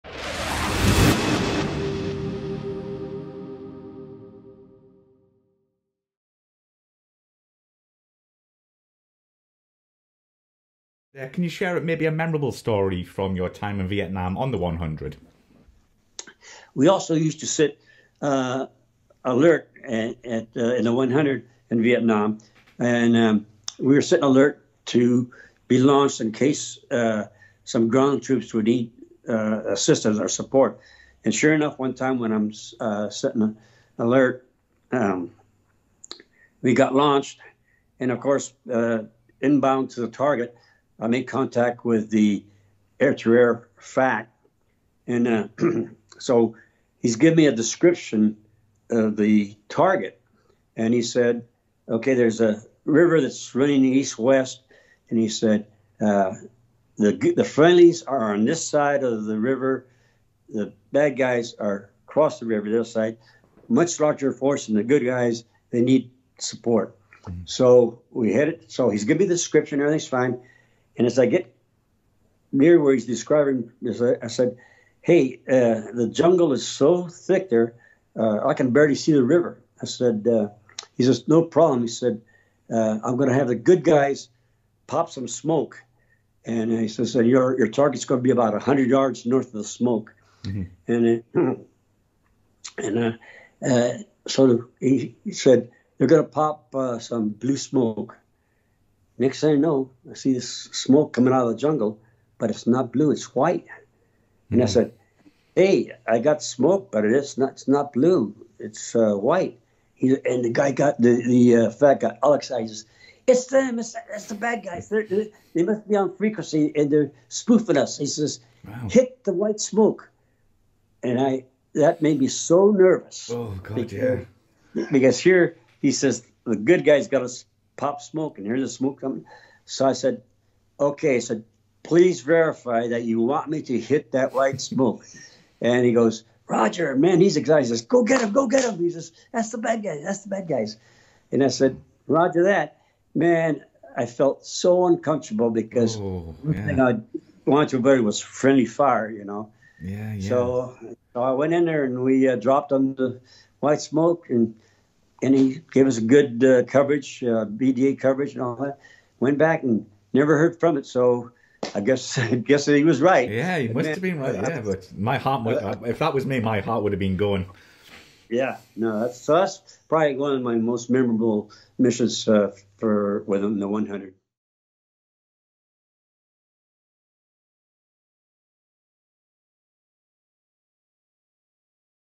F-100 Over Vietnam | Vic Vizcarra
Former F-100 pilot, Vic Vizcarra shares a memorable flight from his time flying the “Hun” in Vietnam.

HUN PILOT: A PILOT'S ACCOUNT OF THE FIRST USAF SUPERSONIC FIGHTER